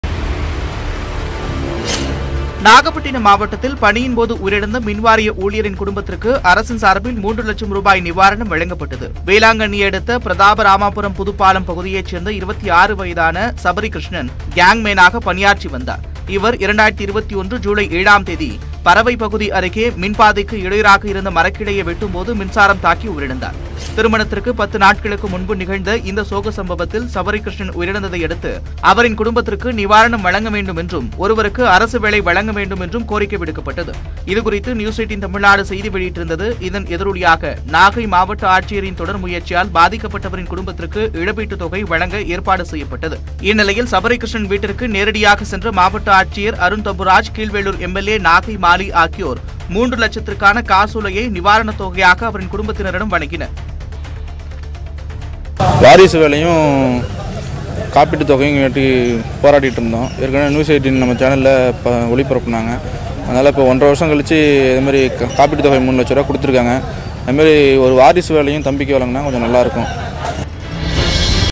News18 Impact | பணியின்போது உயிரிழந்த மின்வாரிய ஊழியரின் குடும்பத்திற்கு ரூ 3 லட்சம் நிதியுதவி
News18 Impact | நாகப்பட்டினம் மாவத்தில் பணியின்போது உயிரிழந்த மின்வாரிய ஊழியரின் குடும்பத்திற்கு ரூ 3 லட்சம் நிதியுதவி | செய்திகளை துல்லியமாகவும் துரிதமாகவும் வழங்கும் தமிழகத்தின் முதன்மை செய்தி தொலைக்காட்சி உங்கள் நியூஸ் 18 தமிழ்நாடு

Sat Nov 19 2022